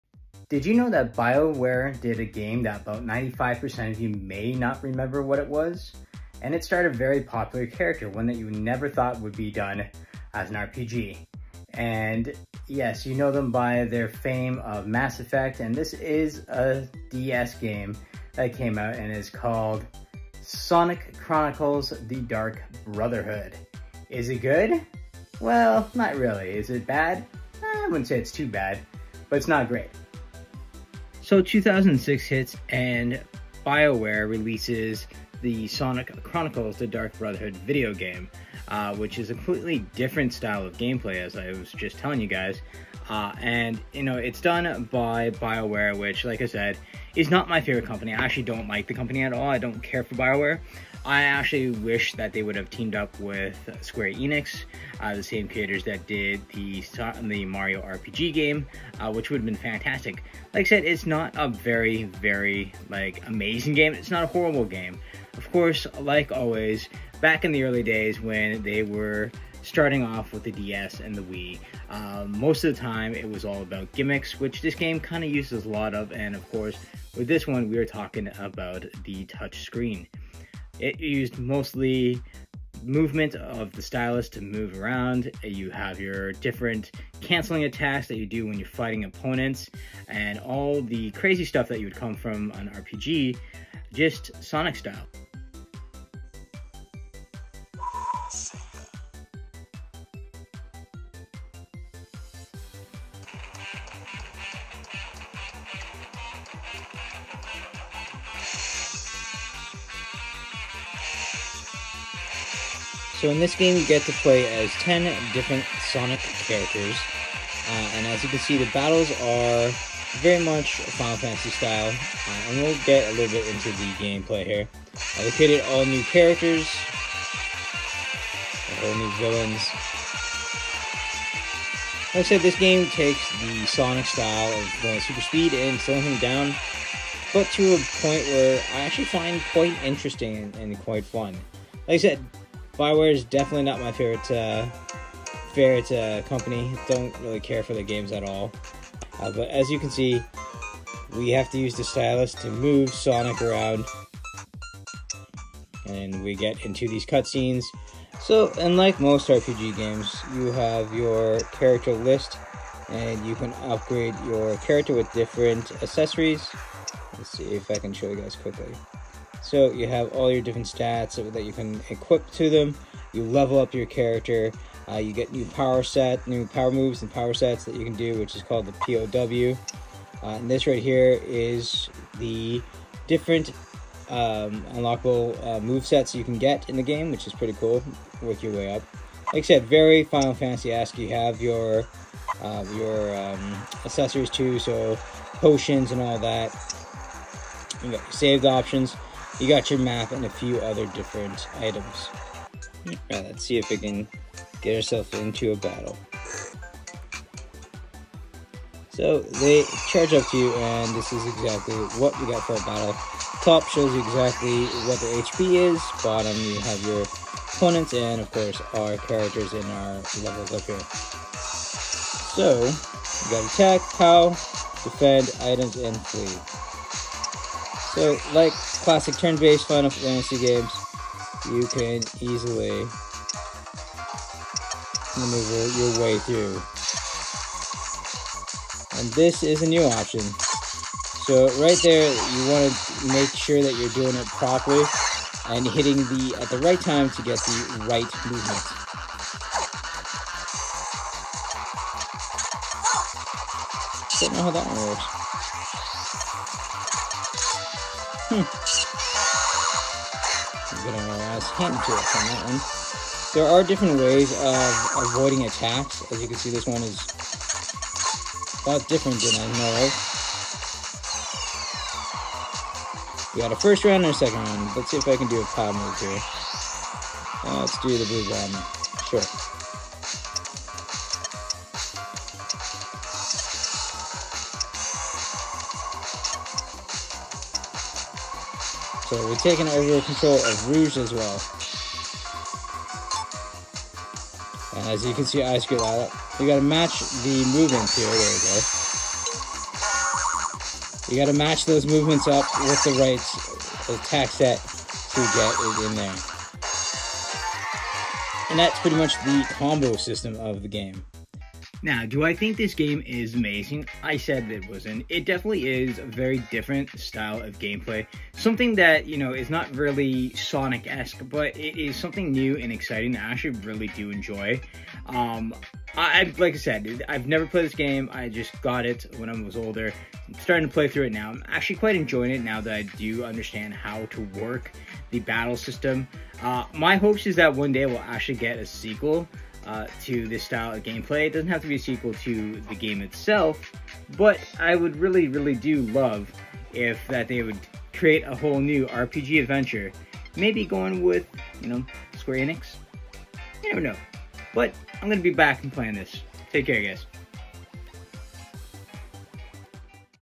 I bet 95% of you don’t remember this game existed.
A forgotten game that I bet you didn’t know was created by BioWare and featured this amazing blue hedgehog, is it good? Is it bad? Believe me when I tell you is different and I kind of like it lol.